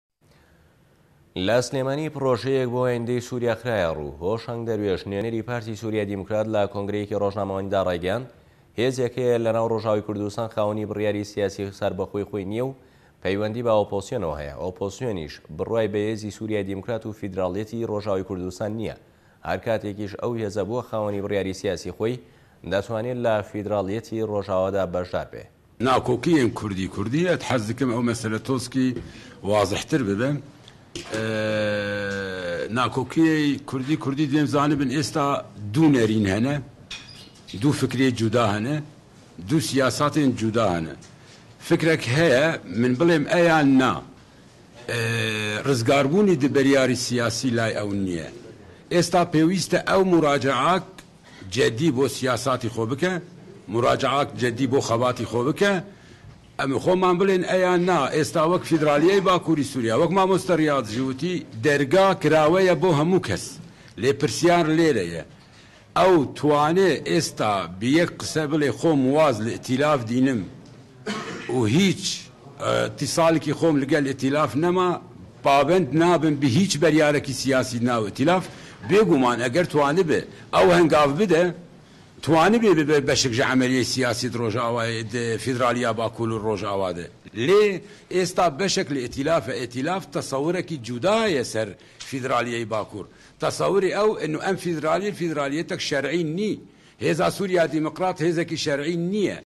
سلێمانی؛ پرۆژه‌یه‌ك بۆ ئاینده‌ی سوریا خرایه‌روو
له‌ شاری سلێمانی پرۆژه‌یه‌ك بۆ ئاینده‌ی سوریا خرایه‌ روو لای خۆیه‌وه‌ هۆشه‌نگ ده‌روێش نوێنه‌ری‌ پارتی سوریای دیموكرات له‌كۆنگره‌یه‌كی رۆژنامه‌وانیدا رایگه‌یاند هێزێك هه‌یه‌ له‌ناو رۆژئاوای كوردستان خاوه‌نی بریاری سیاسی سه‌ربه‌خۆی خۆی نیه‌ و په‌یوه‌ندی به‌ ئۆپۆزسیۆنه‌وه‌ هه‌یه‌، ئۆپۆزسیۆنیش بروای به‌ هێزه‌ی سوریای دیموكرات و فیدراڵیه‌تی رۆژئاوای كوردستان نیه‌، هه‌ركاتێكیش ئه‌و هێزه‌ بووه‌ خاوه‌نی بریاری سیاسی خۆی ده‌توانێت له‌ فیدراڵیه‌تی رۆژئاوا به‌شدار بێت.